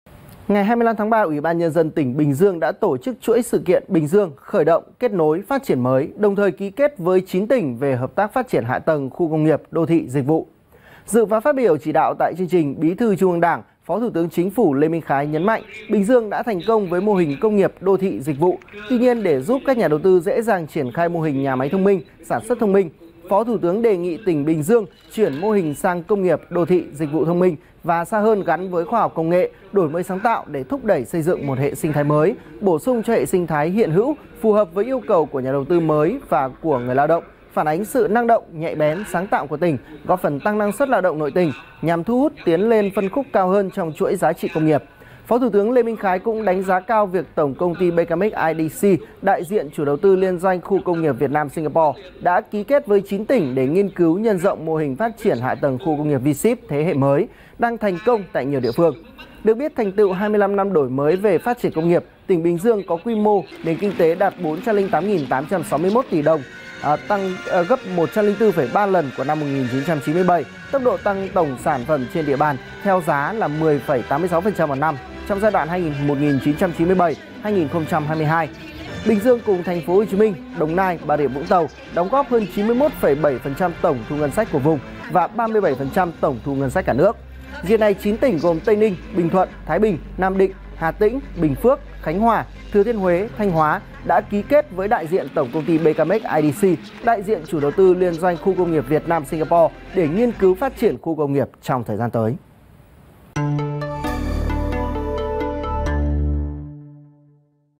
Chương trình “Bình Dương khởi động – Kết nối – Phát triển mới” - VNEWS
VNEWS - Đây là chủ đề sự kiện đánh dấu cho sự phát triển của tỉnh Bình Dương trong hơn 25 năm qua, với thành quả đột phá về cơ sở hạ tầng được đầu tư đồng bộ, chính sách thông thoáng, cởi mở, tư duy phát triển không ngừng đổi mới sáng tạo... vừa được UBND tỉnh tổ chức sáng 25/3.

Mời quý vị và các bạn xem thêm: Nga 'khôn khéo' vượt qua bức tường trừng phạt của phương Tây giúp lấy nguồn vi mạch sản xuất quân sự